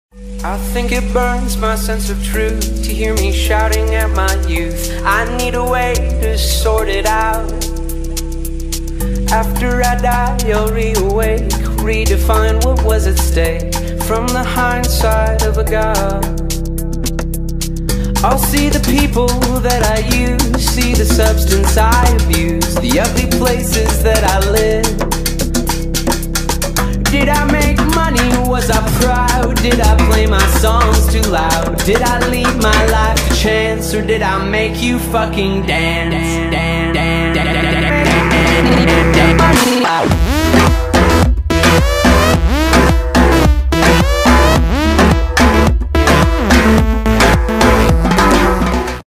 did I play my songs too loud?
[HD!]  Yay this is obviously not me but PursuitForTheBlue!

Sorry for the terrible video, it's pretty watered down until about 00:36... and iMovie is a butt head :P This is the amazing Julia (PursuitForTheBlue) and she is seriously one of my favorite editors on here and seriously deserves more subscribers (not that I'm much help lol).  You guys should seriously check her out:

Anyways I haven't been uploading anything with my own clips because frankly I haven't been riding much lately and if I had it's usually really bad riding or not getting clips... and I don't really like editing to my own clips lol